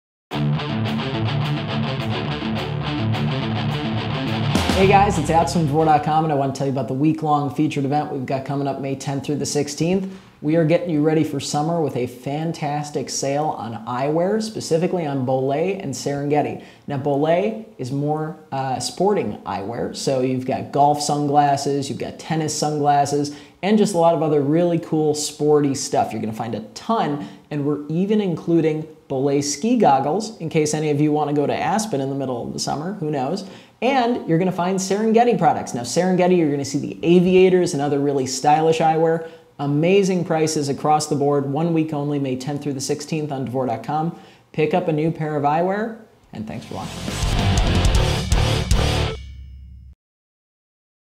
Bolle & Serengeti Event - Dvor.com
Check out this event 5/10-5/16/15 at Dvor.com now!

This week's featured event is on Bolle & Serengeti eyewear. We have tons of models that are both stylish and practical, sporty and fashionable. Protect your eyes in these summer months and check out this event.

Also, there's a ton more events just like this one at Dvor.com, check it out!